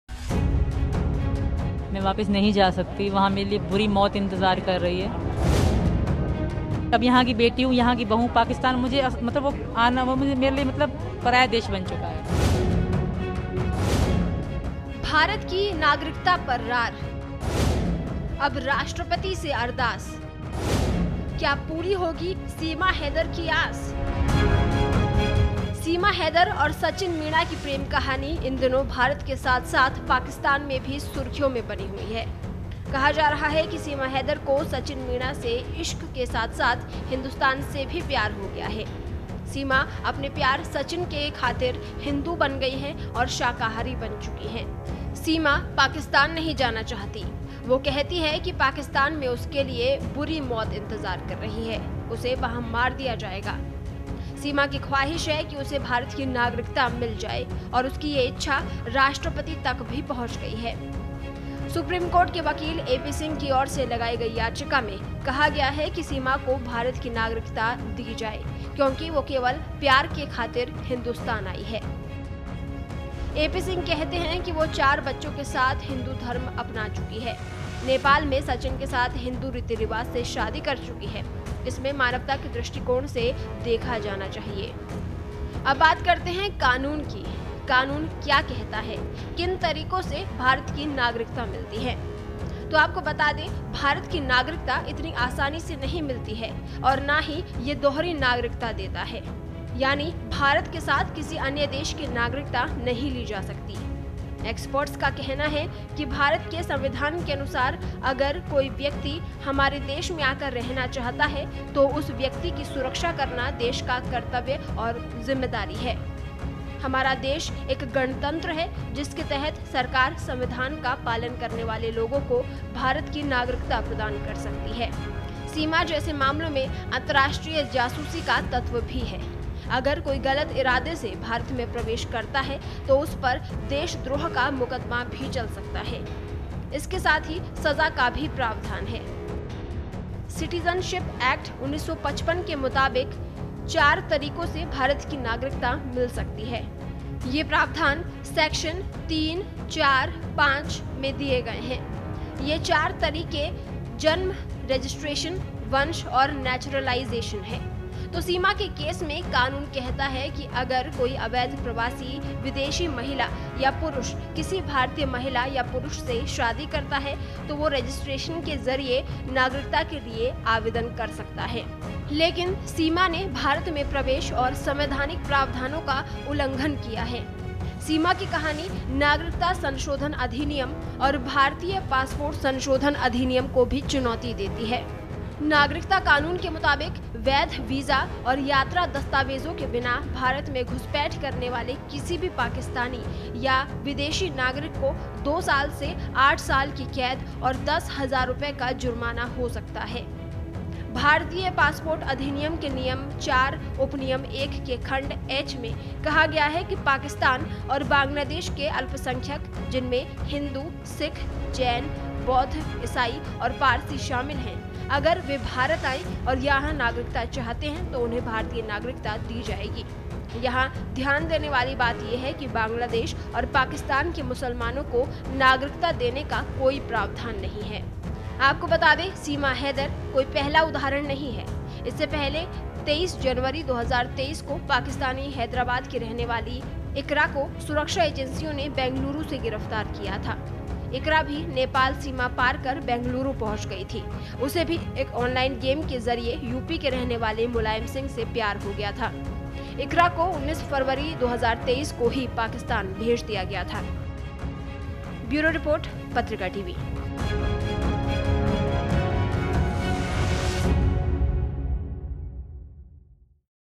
SEEMA HAIDER ने अपना लिया आखिरी रास्ता । SEEMA HAIDER NEWS । SEEMA HAIDER LATEST NEWS
SEEMA HAIDER PAKISTAN NEWS: क्या होगा SEEMA HAIDER का? अपना लिया आखिरी रास्ता । SEEMA HAIDER NEWS । SEEMA HAIDER LATEST NEWS

सीमा हैदर और सचिन मीणा की प्रेम कहानी इन दिनों भारत के साथ-साथ पाकिस्तान में भी सुर्खियों में बनी हुई है। कहा जा रहा है कि सीमा हैदर को सचिन मीणा से इश्क के साथ-साथ हिंदुस्तान से भी प्यार हो गया है। सीमा पाकिस्तान नहीं जाना चाहती। वह कहती है कि पाकिस्तान में उसके लिए बुरी मौत इंतजार कर रही है। उसे वहां मार दिया जाएगा। सीमा की ख्वाहिश है कि उसे भारत की नागरिकता मिल जाए।